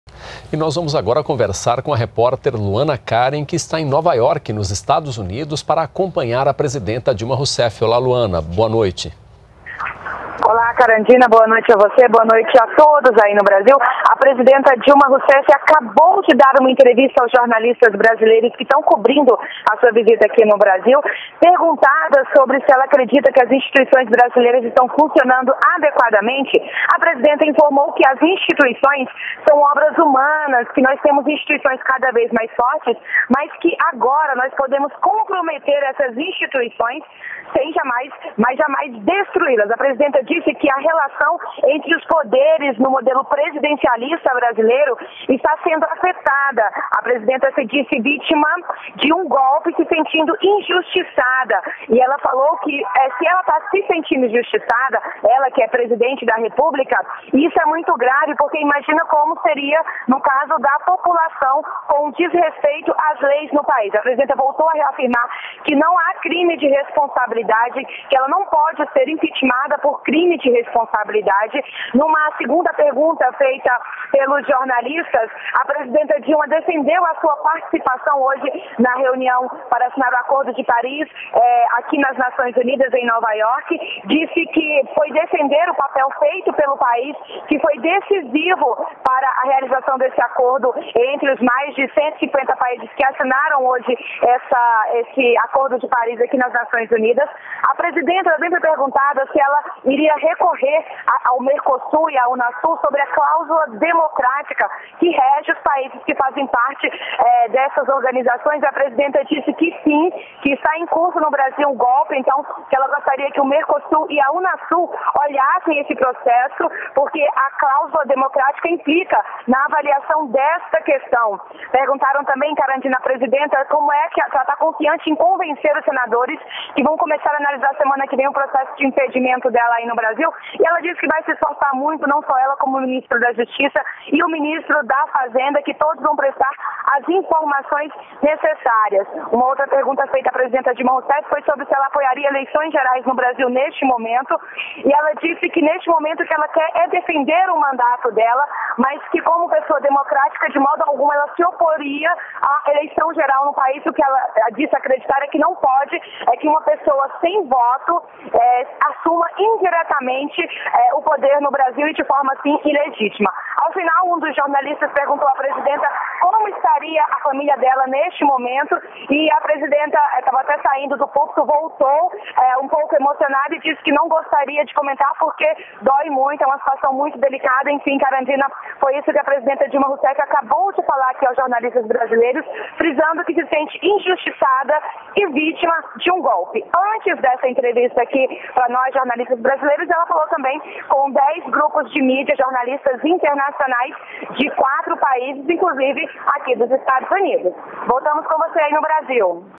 Dilma Rousseff concede entrevista para jornais internacionais em Nova Iorque
NBR NOTÍCIAS - 22.04.16: Durante viagem aos Estados Unidos, Dilma Rousseff concedeu entrevista para jornais internacionais. Na conversa, Dilma falou sobre o momento político no país e afirmou que se sente injustiçada e vítima de um golpe.